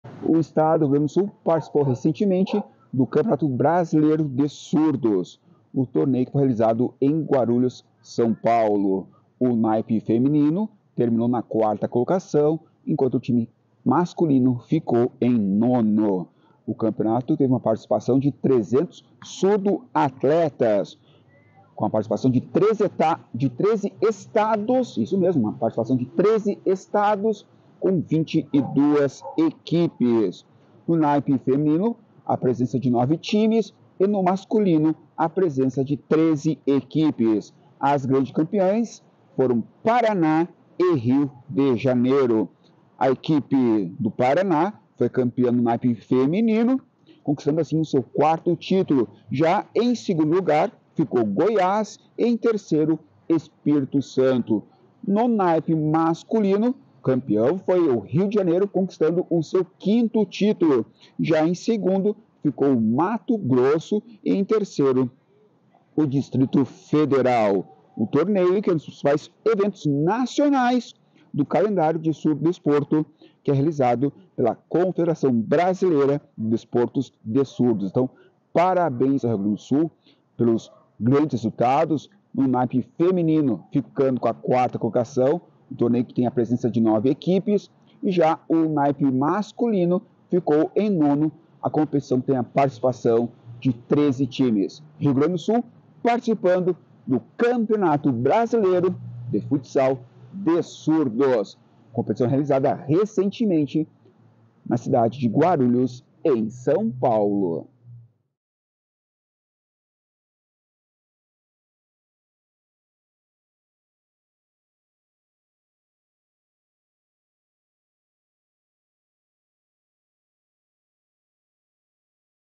Rio Grande do Sul conquista resultados positivos no Campeonato Brasileiro de Futsal de Surdos 2024
O Rio Grande do Sul participou do Campeonato Brasileiro Caixa de Futsal de Surdos 2024. No feminino, a equipe gaúcha terminou em 4º lugar e no masculino ficou em 9º.

Com a participação de 300 surdoatletas, o evento reuniu 22 equipes de 13 estados, sendo 9 times na categoria feminino e 13 na categoria masculino. As federações do Paraná e do Rio de Janeiro foram as grandes campeãs.

A equipe da Federação Desportiva de Surdos do Paraná (FDSP/PR) foi a grande campeã na categoria feminino, conquistando seu 4º título. A equipe da Federação Goiana de Desportos dos Surdos (FGDS/GO) ficou em 2º lugar e a equipe da Federação Desportiva dos Surdos do Espírito Santo (FDSES/ES), em 3º lugar.

Já no masculino, a Federação Desportiva dos Surdos do Estado do Rio De Janeiro (FDSERJ/RJ) conquistou seu 5º campeonato. O 2º lugar ficou com a equipe da Federação Desportiva de Surdos de Mato Grosso (⁠FDSMT/MT) e o 3º lugar com a equipe da Federação Brasiliense Desportiva dos Surdos (FBDS/DF).

O torneio é um dos principais eventos nacionais do calendário do surdodesporto do Brasil, o campeonato, é realizado pela Confederação Brasileira de Desportos de Surdos (CBDS).

Créditos de imagem: Lidiana Matos | CBDS